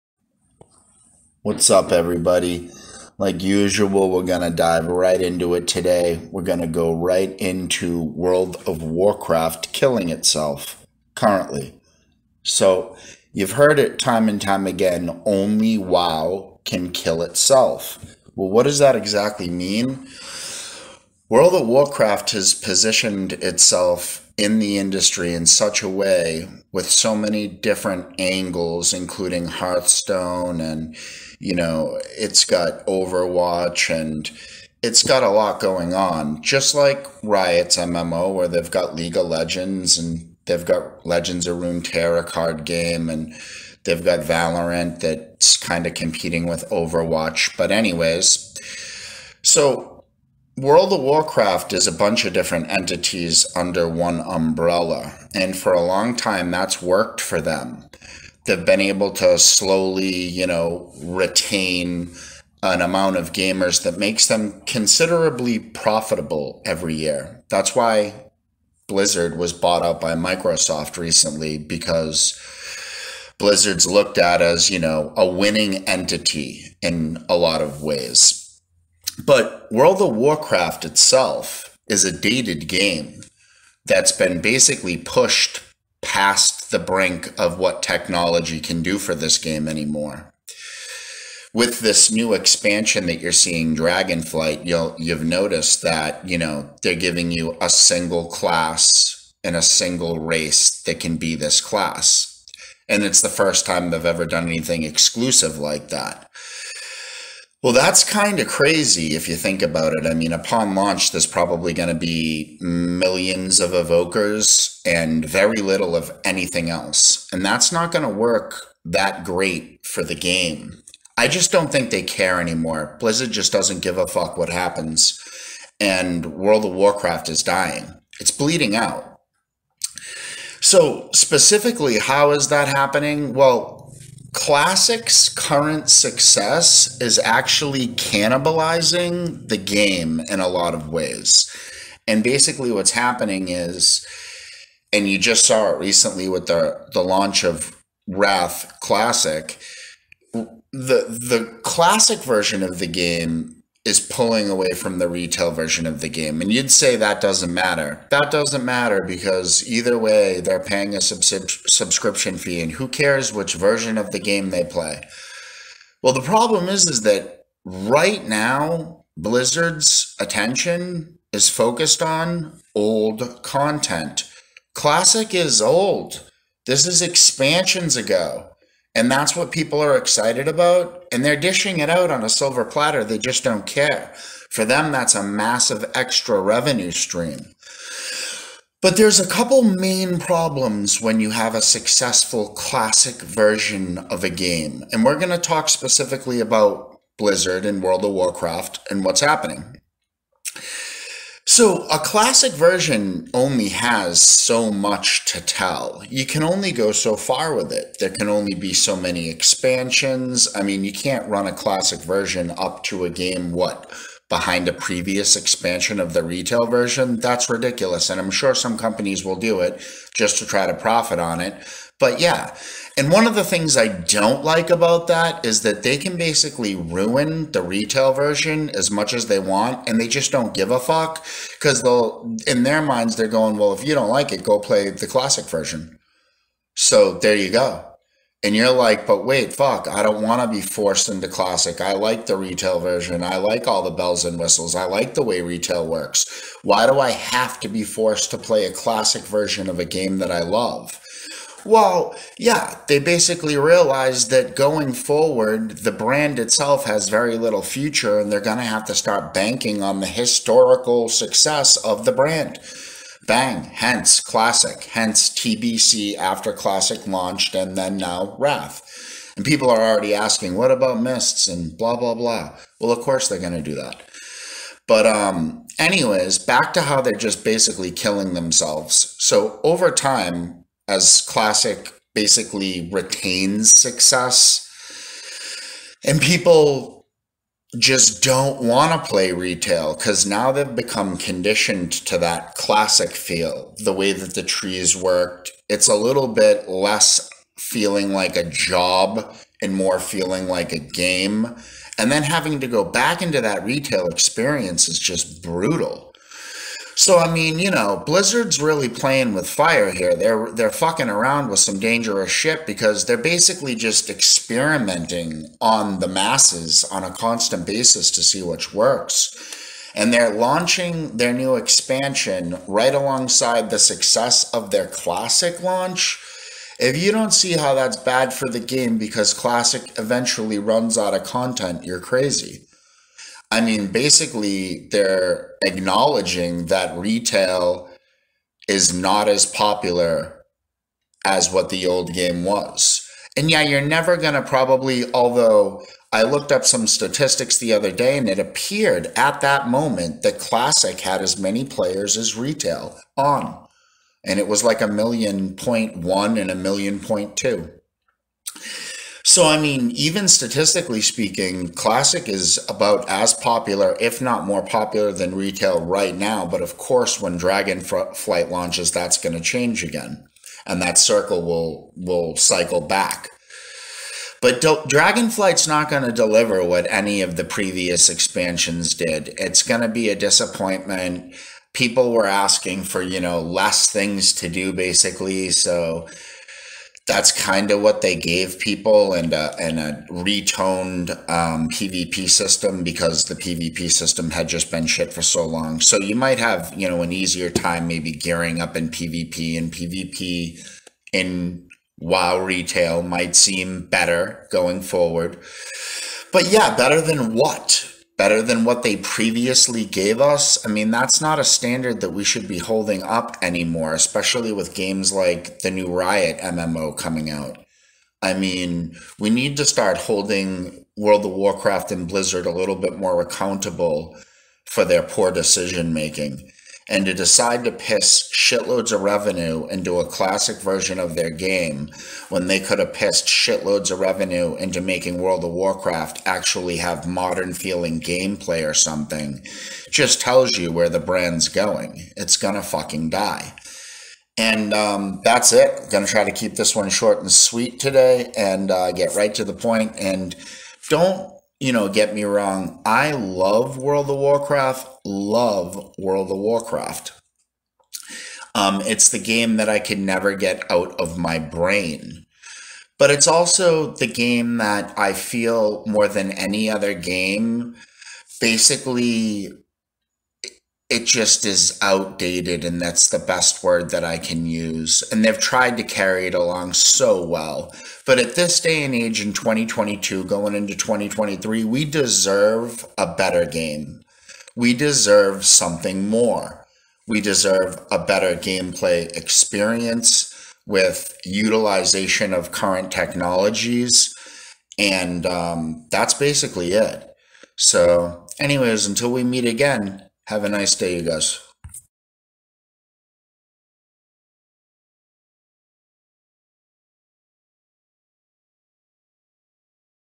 Forced Into Classic: Wow Killing Itself
Podcast style video today with audio only. Short clip where I discuss how World of Warcraft is cannibalizing its own brand by the success of wowclassic WOTLK. Over the next few years wow will run out of Classic content to produce and be left using what they built for their future...Nothing!